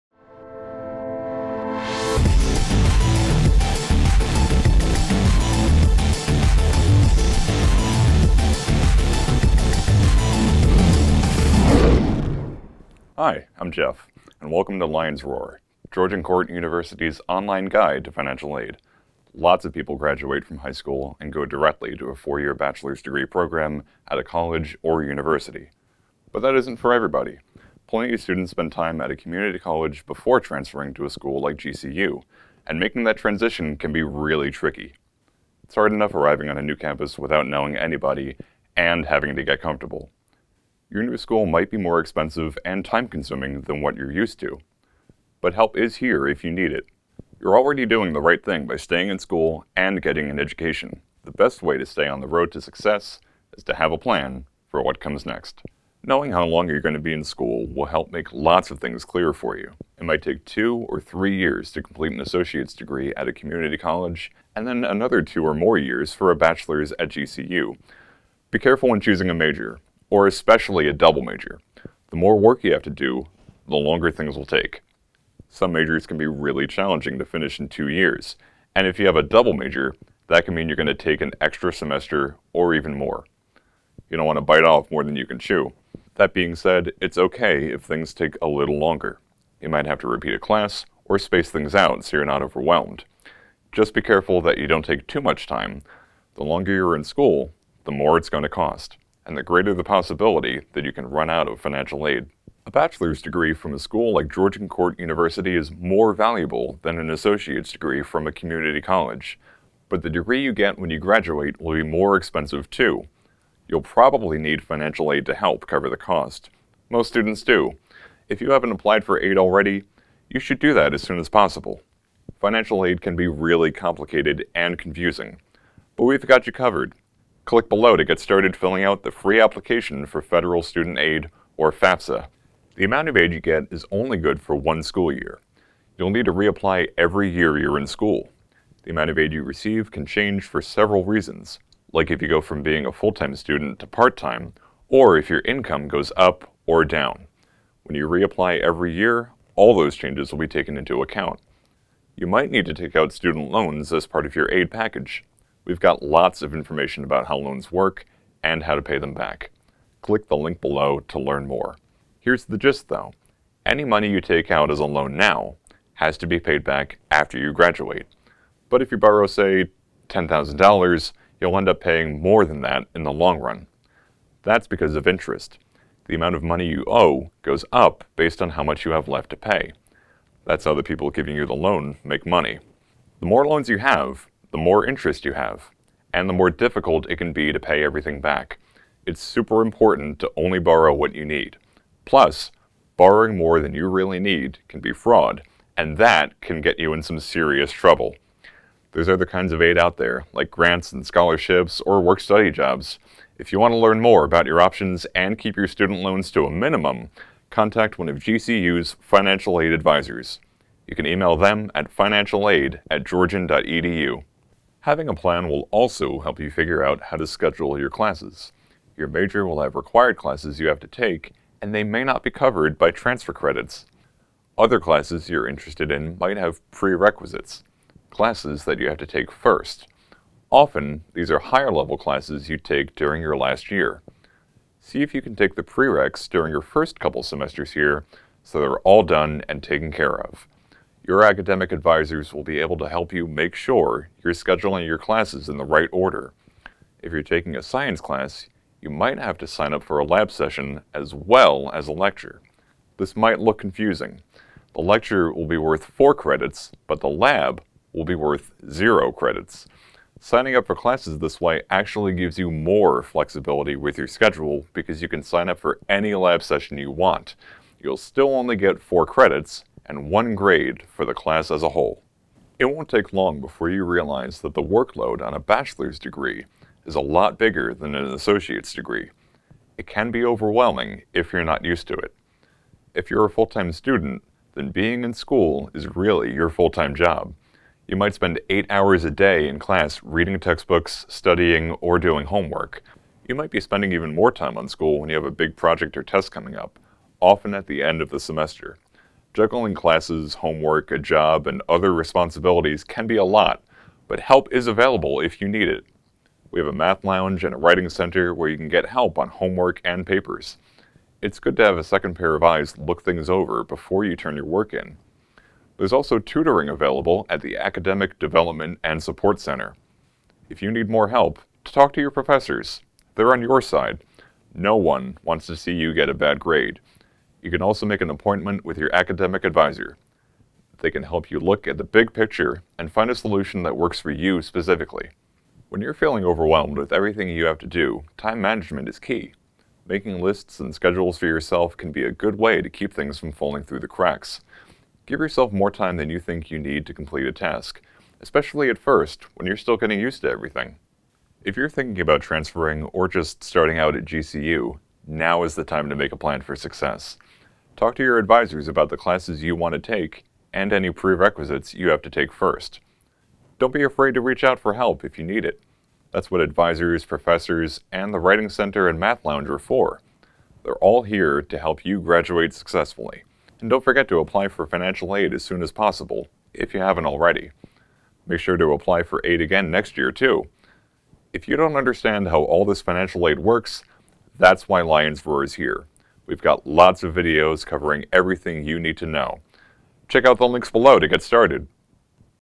1 Transfer Students
Did you transfer to GCU from another school? Welcome! We have some tips to help you get settled here on campus.